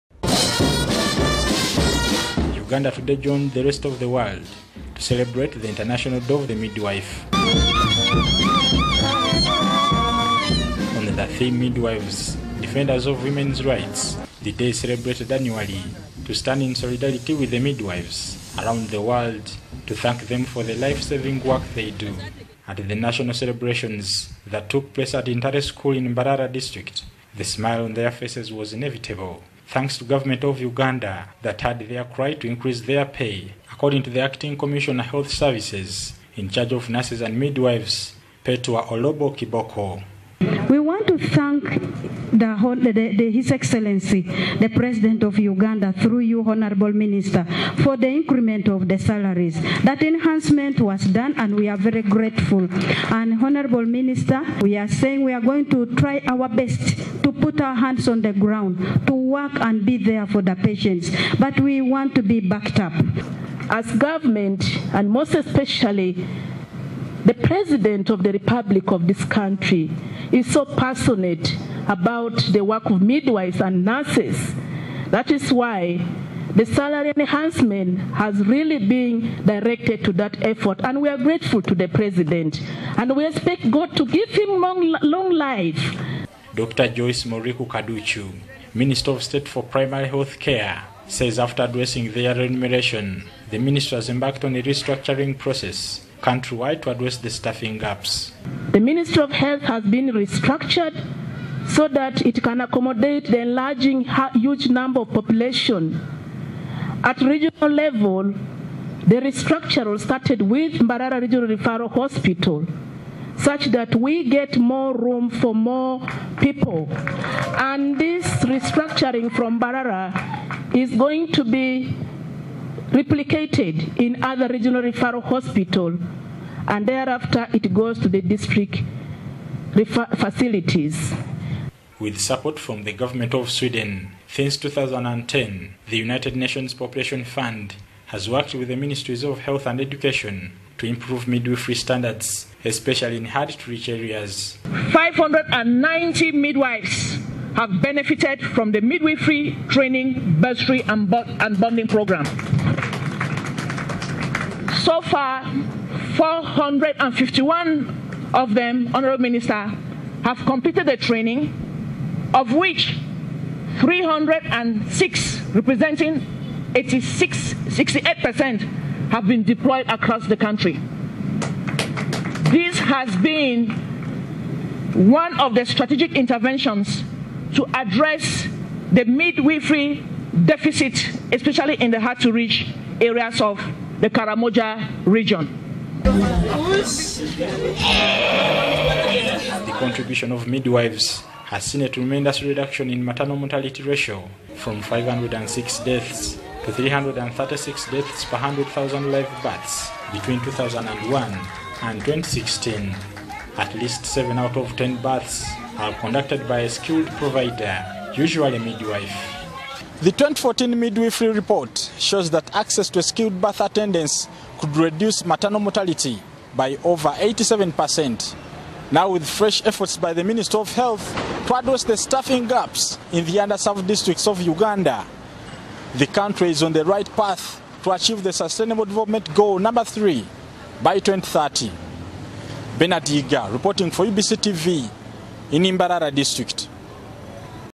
Government embarks on a restructuring process to address the staffing gaps in the health sector.
Government has embarked on a restructuring process in hospitals to address the staffing gaps in the health sector.
This was revealed by the State Minister for health in charge of primary health care Dr Joyce Moriku Kaducu while officiating at the international day of the midwife held in Mbarara district where midwives thanked government for increasing their salaries.